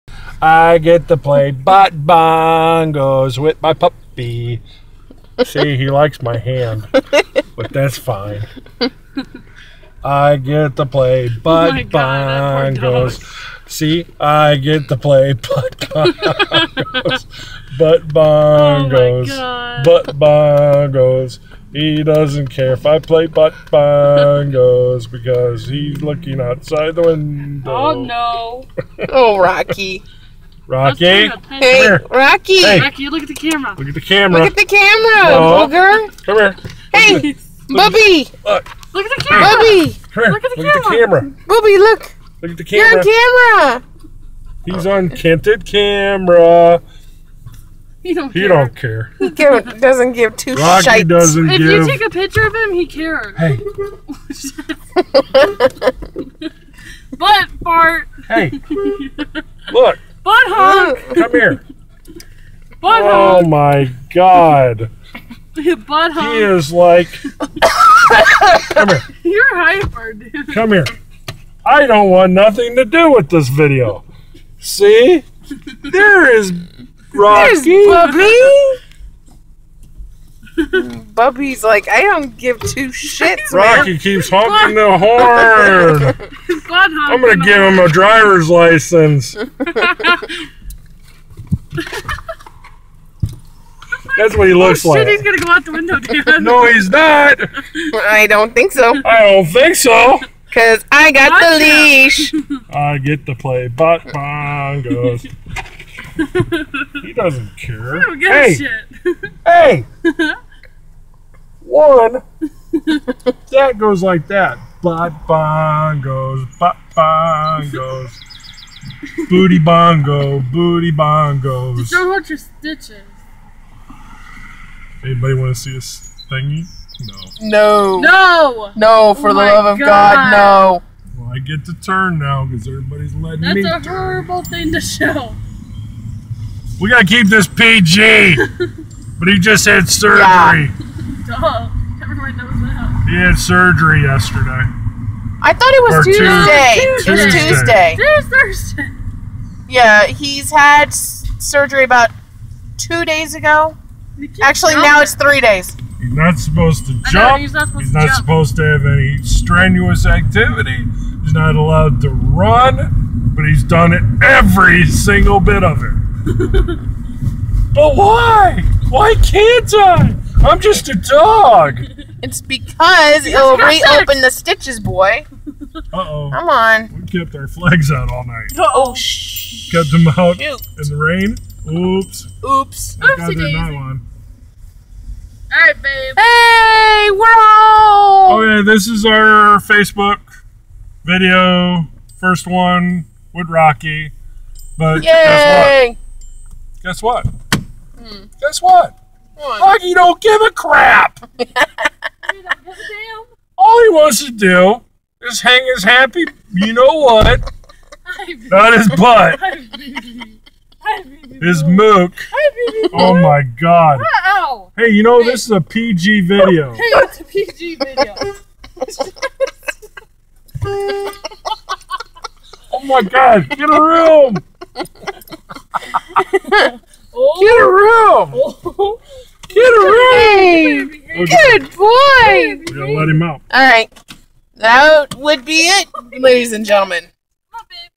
Butt Bongos with Labrador Puppy
playinh bongos on my puppy Rocky